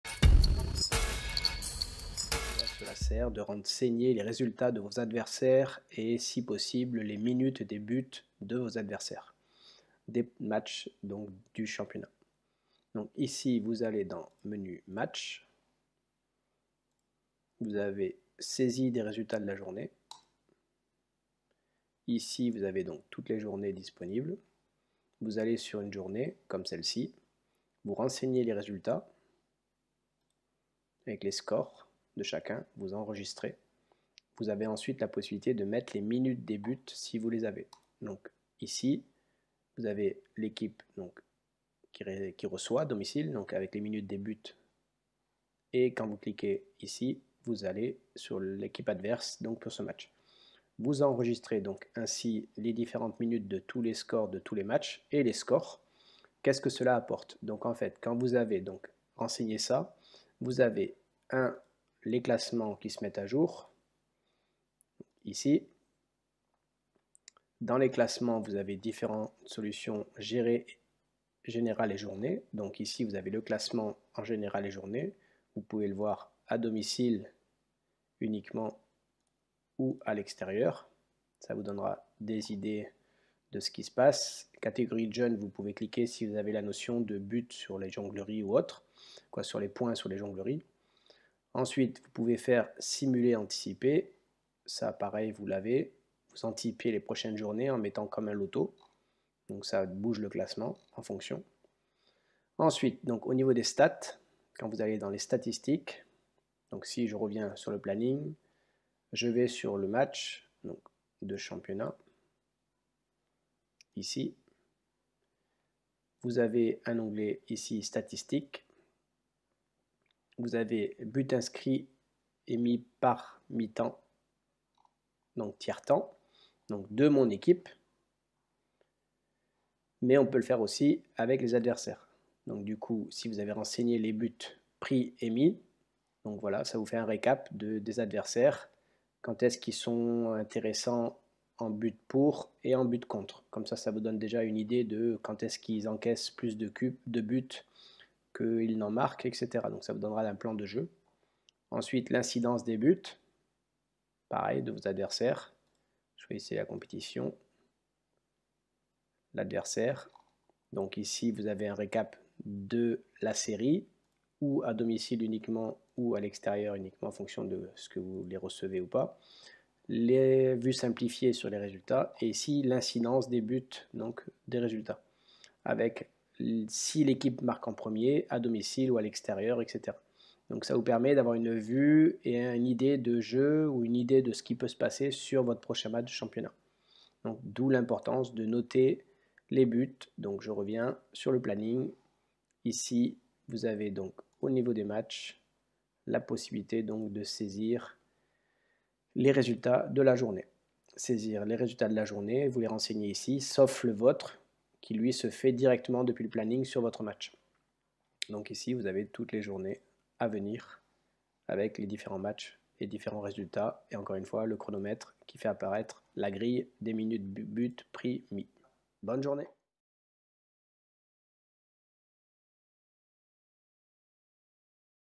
Tuto: Résultats des adversaires
Comment saisir les résultats de vos adversaires et pourquoi

credit FiftySounds.